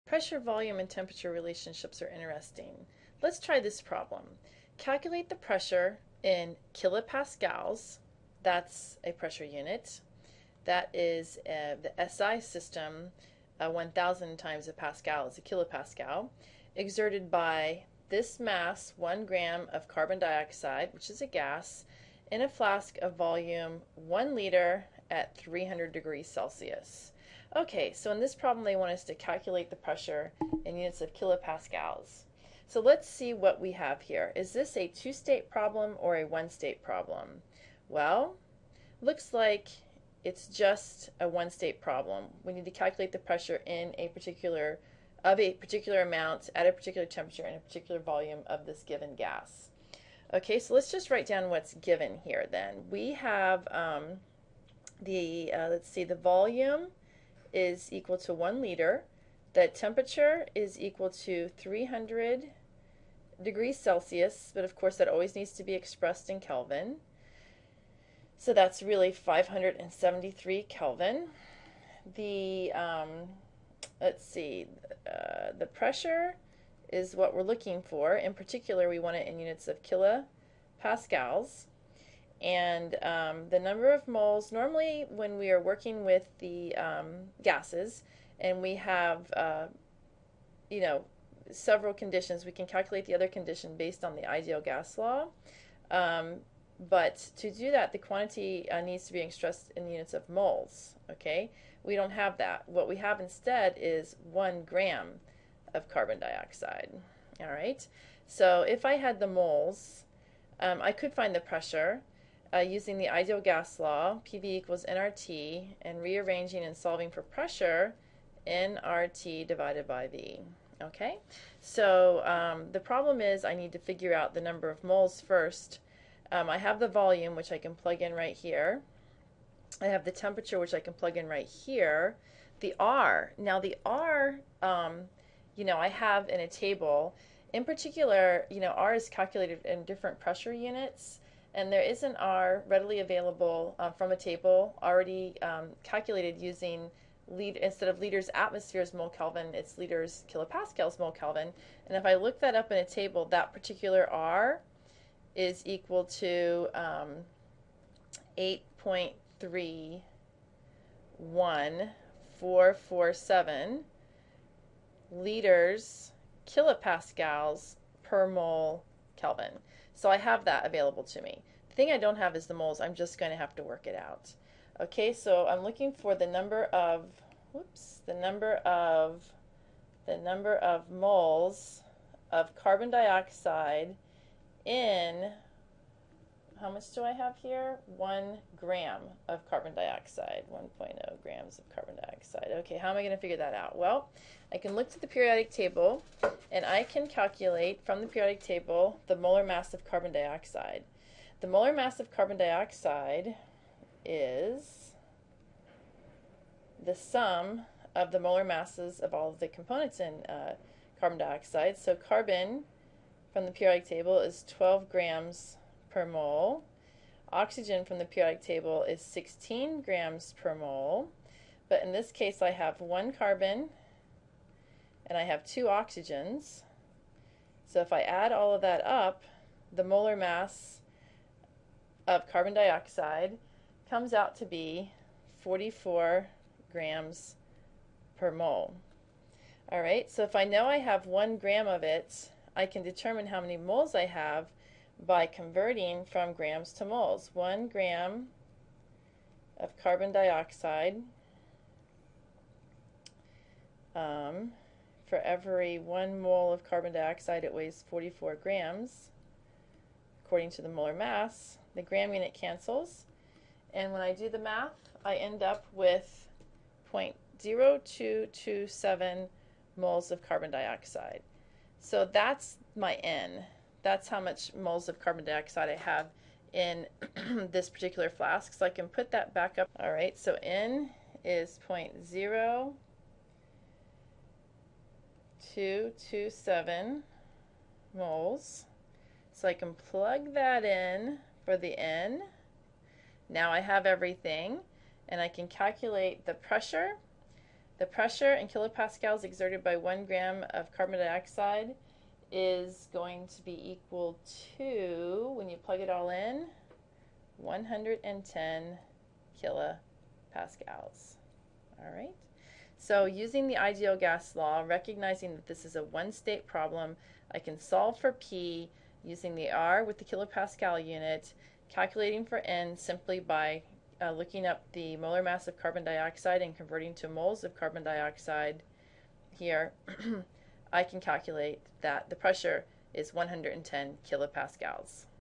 Ideal Gas Law Example
This short video works anexample of an  Ideal Gas Law problem in which the amount of the gas is given in grams instead of moles.